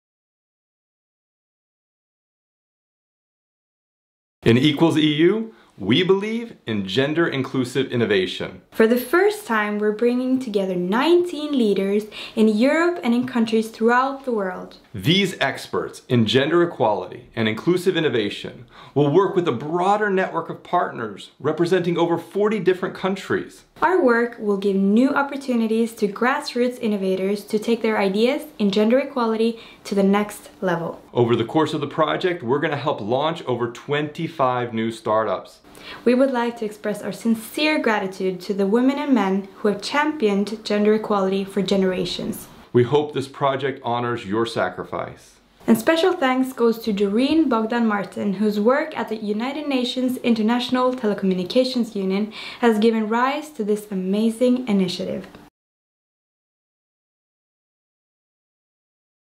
What is EQUALS-EU?
Project initiator and leader Dr. Anthony Giannoumis and Helene Rogne, project coordinator, from Oslo Metropolitan University present you a framework of the 3 year project that champions gender equality and supported by EU Horizon 2020 programme.